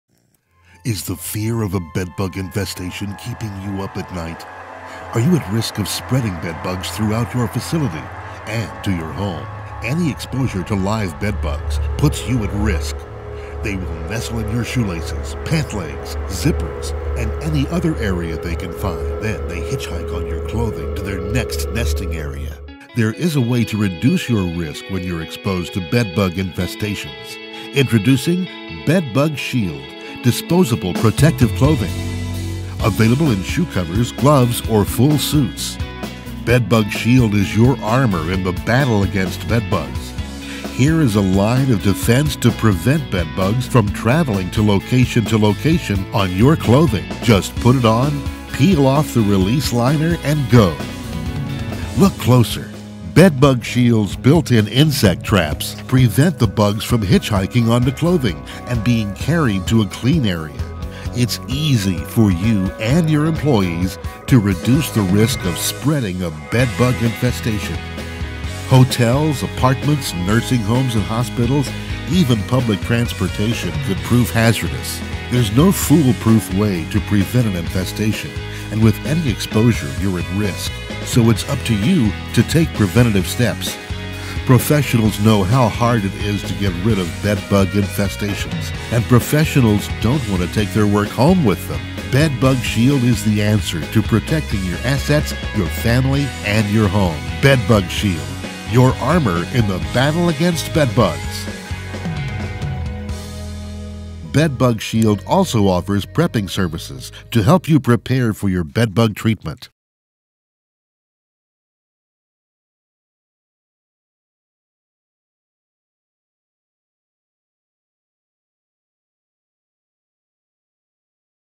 Cincinnati Video Production - Bed Bug Shield Products
Here's a unique product that lets professionals and anyone who may be exposed to bed bugs work without spreading the risk of exposure.